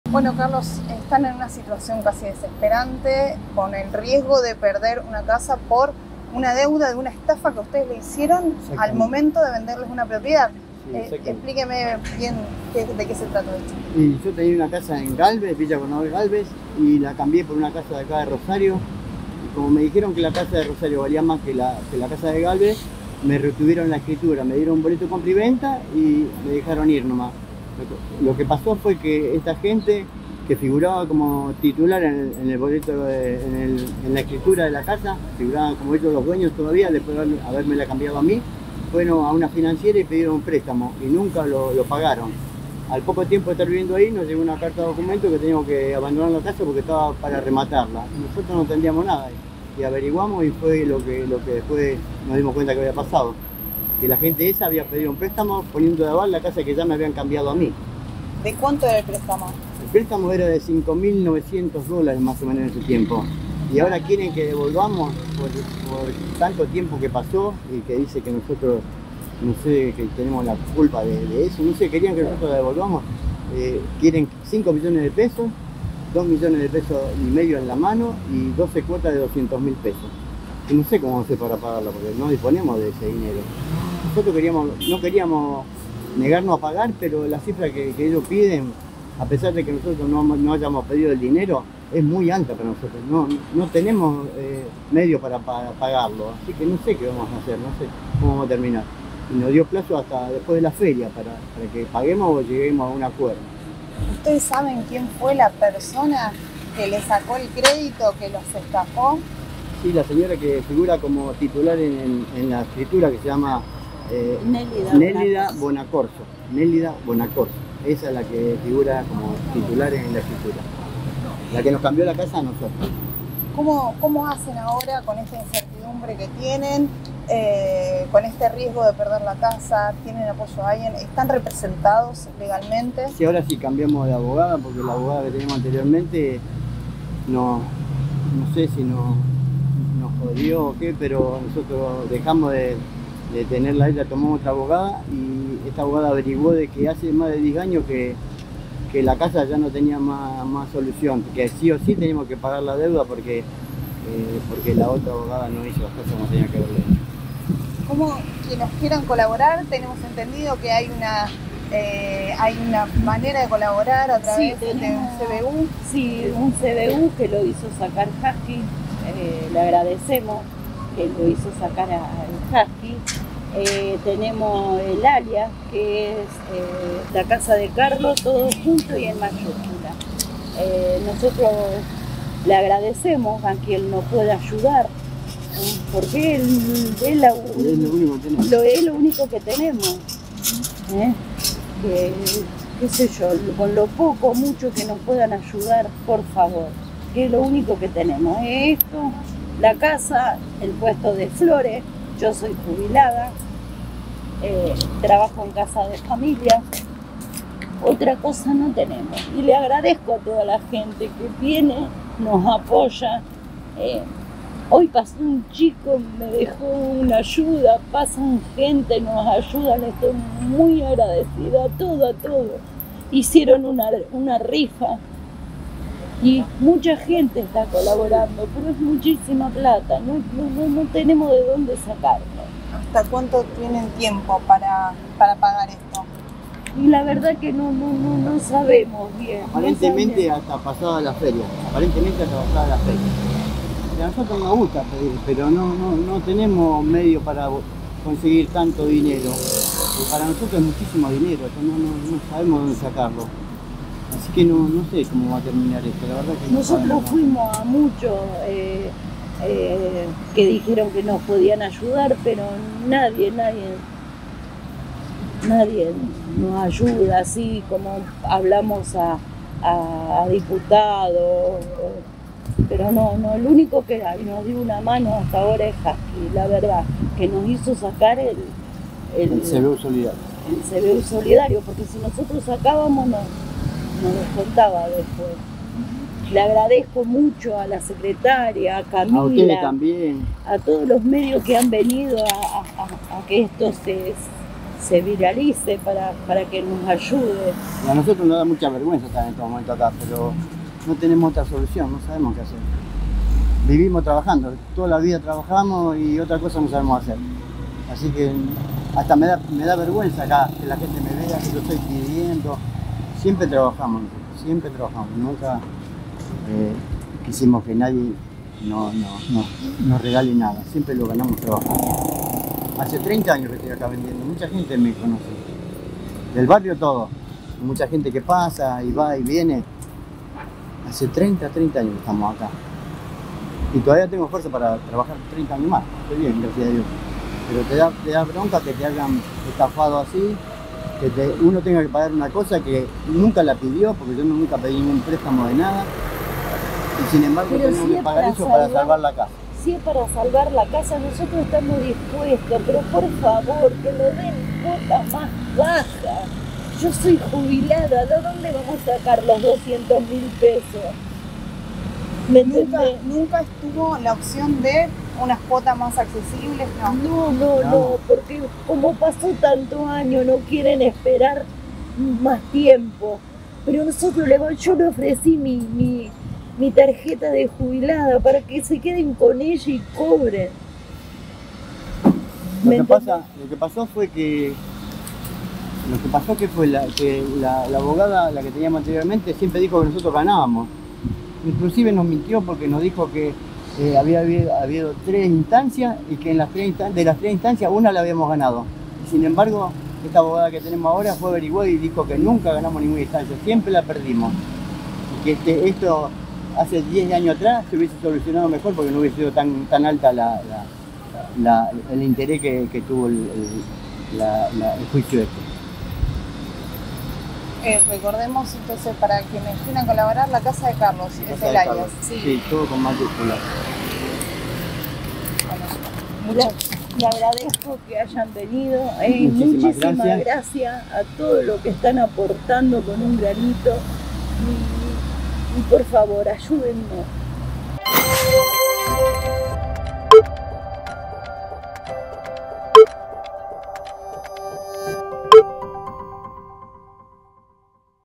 Los estafaron y ahora pueden perder la casa en la que viven desde hace 21 años
Carlos y Susana compraron una casa y les retuvieron la escritura. En ese momento la titular sacó un crédito y puso la propiedad como garantía. Ahora les exigen el pago de 5 millones de pesos. Piden colaboración para no perder su hogar.